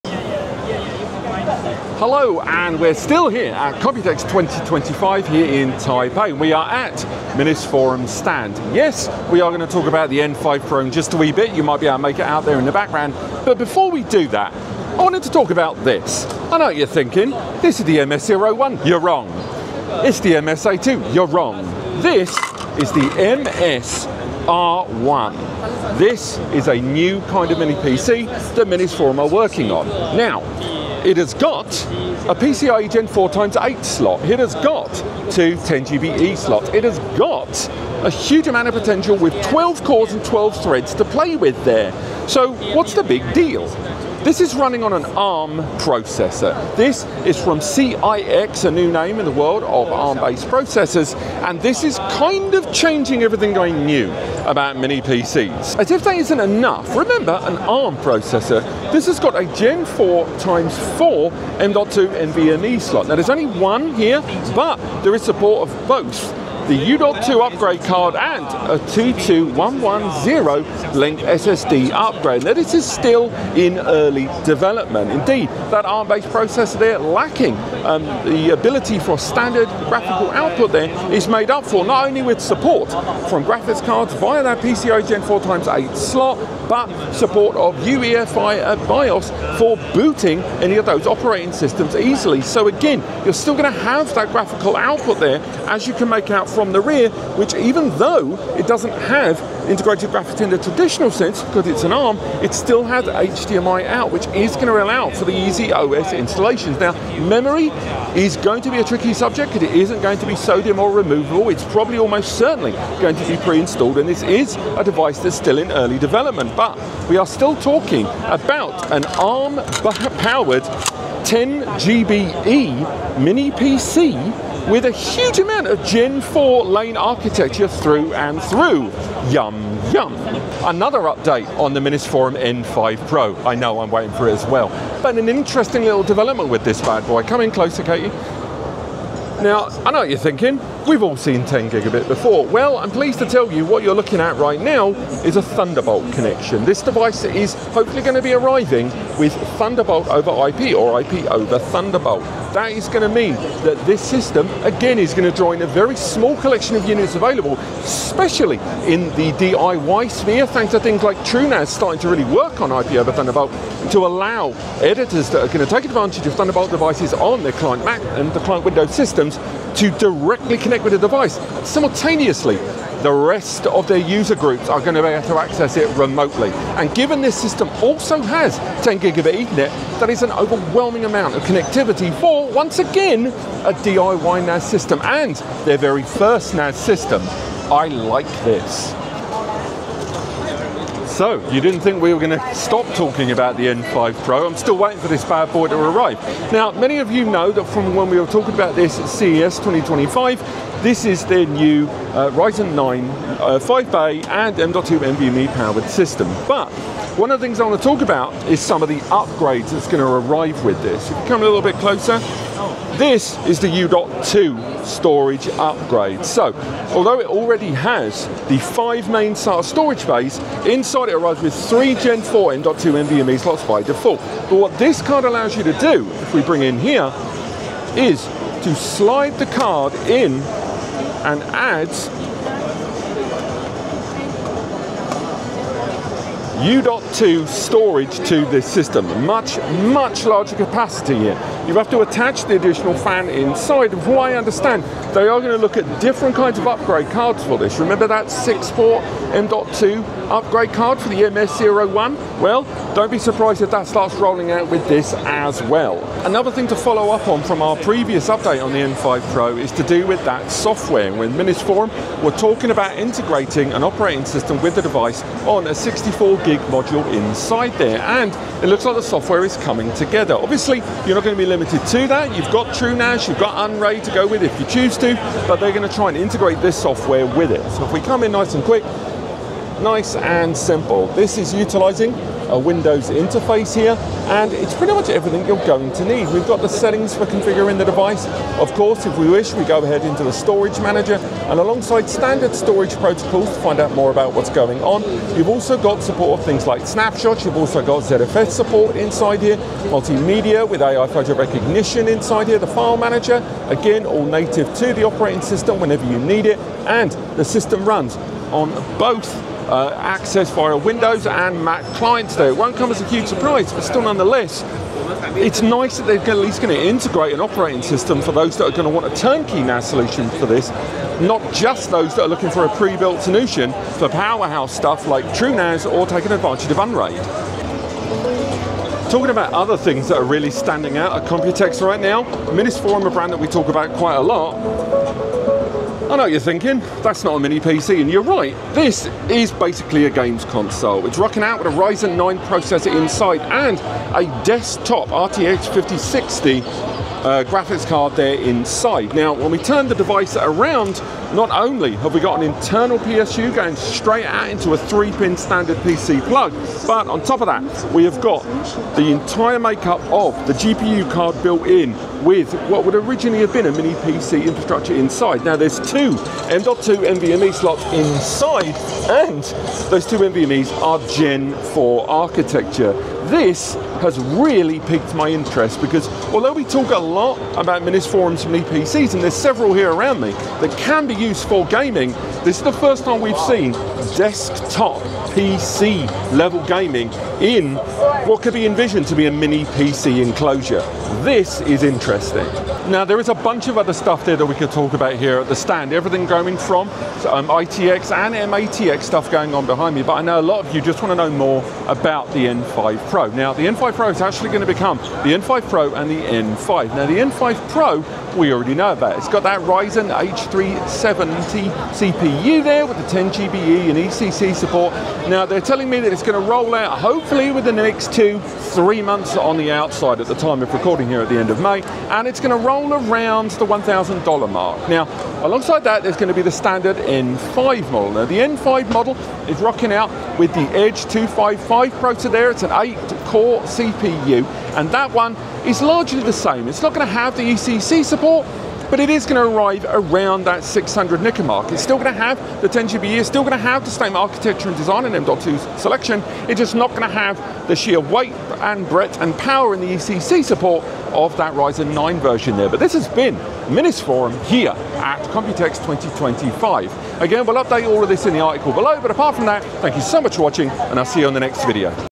Minisforum MS-R1 and N5 Pro NAS Updates
Minisforum N5 Pro NAS - Is it too Early for NAS of the Year (article)
Written article (more pictures + tests) of the Minisforum 6x M.2 Upgrade Card
MINISFORUM MS-01 Workstation on Amazon

Video Chapters
00:00 – Intro: Back at the Minisforum Stand at Computex 2025
00:18 – Introducing the MS-R1: ARM-Based Mini Workstation
00:49 – MS-R1 Hardware Specs: PCIe Gen4 x8, 2x10GbE, NVMe + U.2
01:13 – MS-R1: Boot Support via UEFI + HDMI on ARM CPU
01:45 – MS-R1 Memory & Development Status
02:09 – N5 Pro Update: Thunderbolt over IP Support Incoming
02:28 – N5 Pro Connectivity: 10GbE + Thunderbolt for Direct Client Access
03:15 – N5 Pro U.2 Upgrade Card: Higher Capacity Options Coming
03:36 – Card Installation, Fan Add-On, and Future Expansion Potential
04:22 – Embedded OS on 64GB Module + Optional TrueNAS/Unraid Support
04:38 – N5 Pro GUI OS Overview: ZFS, Snapshots, AI Media Indexing
05:22 – Minisforum Ryzen Gaming Console – RTX 560 + Ryzen 9 Desktop
06:06 – Dual M.2 NVMe, Gen4 – A New Form Factor for Mini Gaming PCs
06:33 – N5 Series Split: N5 Pro vs Standard N5 Model Confirmed
06:49 – N5 Pro: Ryzen 9 H370 + ECC, 10GbE, ~$1000 Price
07:08 – N5: Ryzen Edge 255, No ECC, ~$600 Price, Same Connectivity
07:28 – Wrap-Up: Minisforum 2025 Lineup Expands and Evolves

Jonsbo N2 vs N3 NAS Case - Get It Right First Time!

Looking for deals and offers on all things NAS?

The NAScompares Podcast - Let's Talk Data
Amazon Podcasts